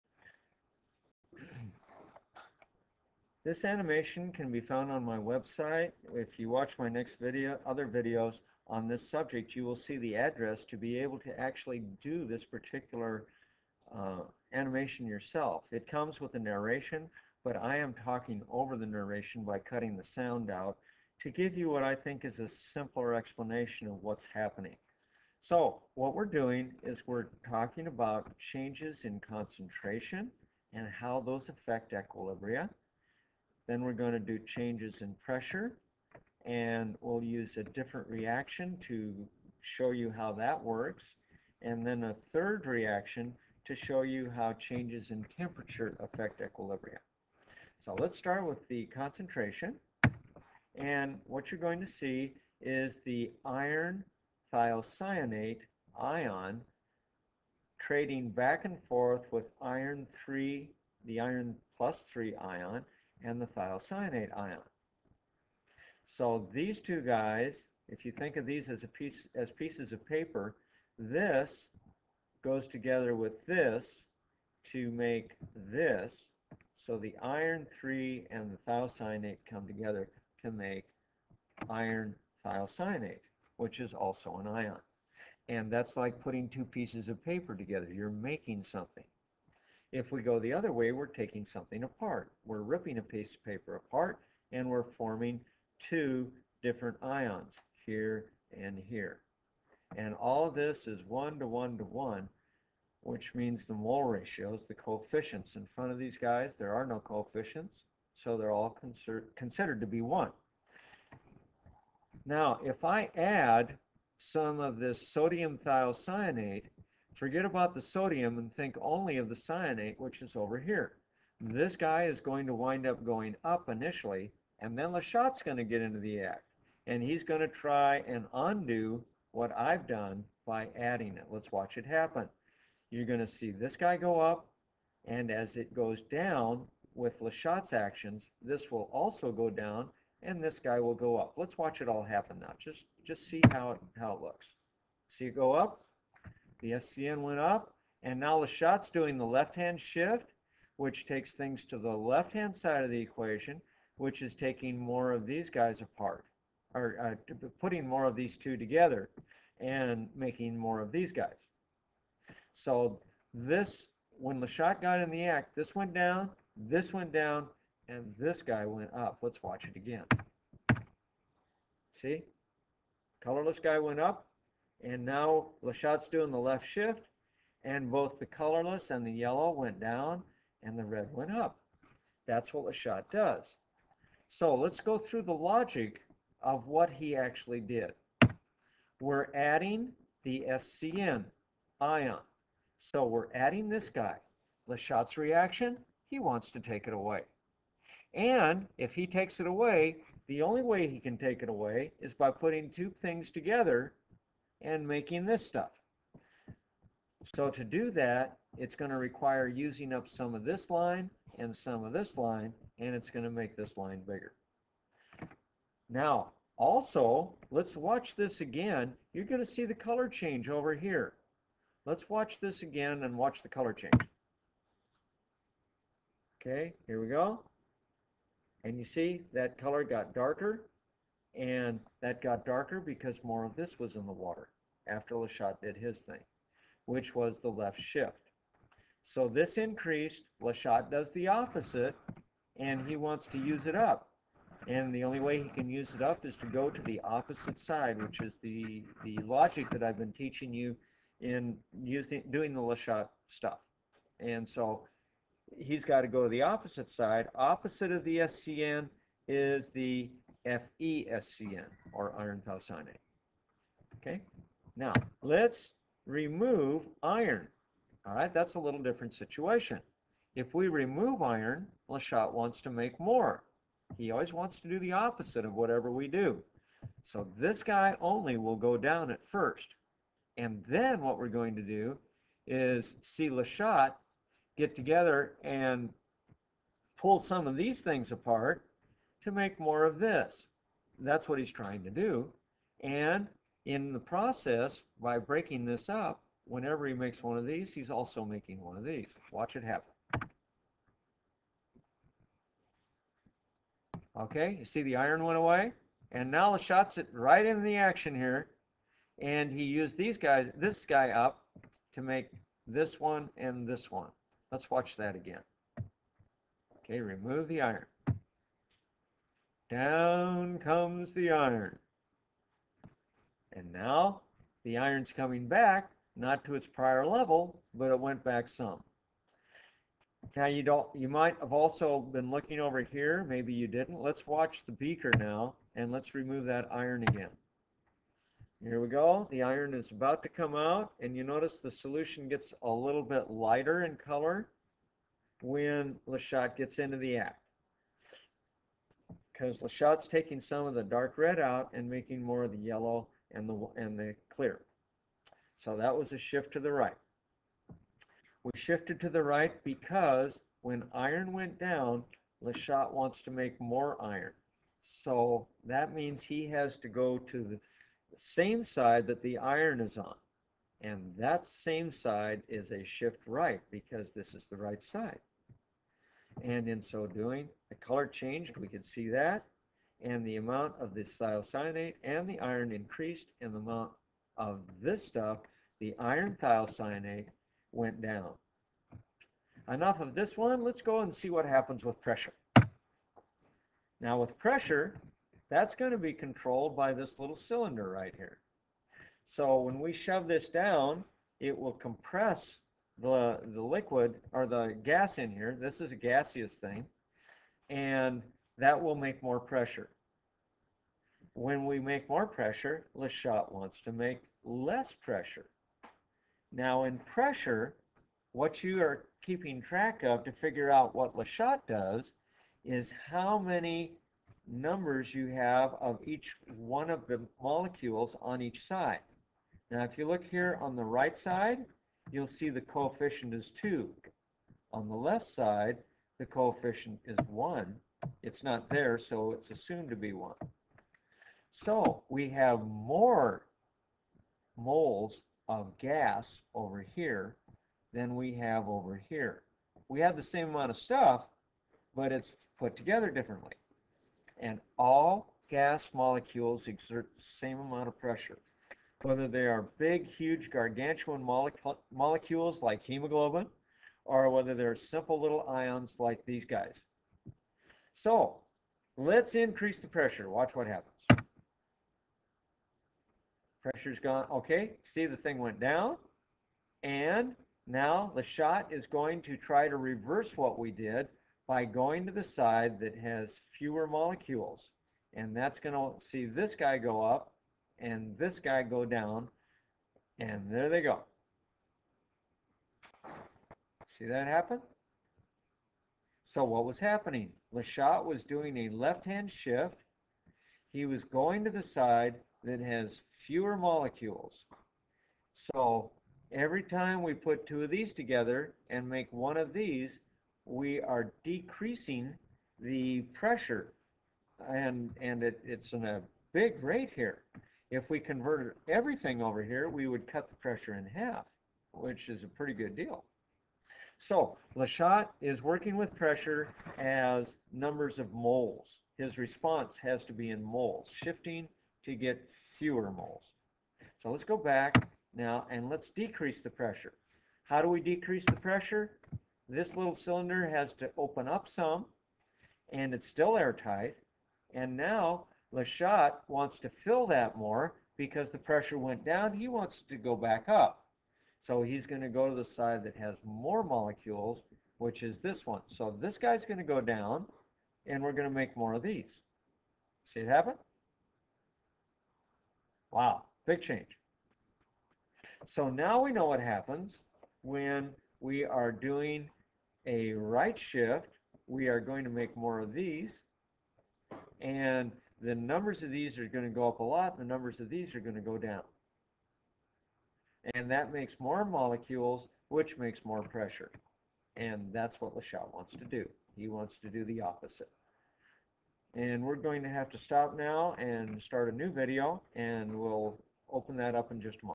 LeChatelier's Principle - 3 conditions that affect equilibria - Part 1
The animation being used here was made by McGraw-Hills and helps you to visualize what is happening when temperature, pressure, or concentration change.  There are three different equations used, each one lends itself to a particular type of change.  This is one aqueous solution where we add or subtract different molecules and what LeChatelier does in response.  The logic you need to use is:

1.  figure the stress
2.  figure what LeChatelier will do in response to the stress
3.  decide whether LeChatelier will move to the SAME or DIFFERENT side of the equaton that the substance is on.
4.  then you can clearly see what the shift will be.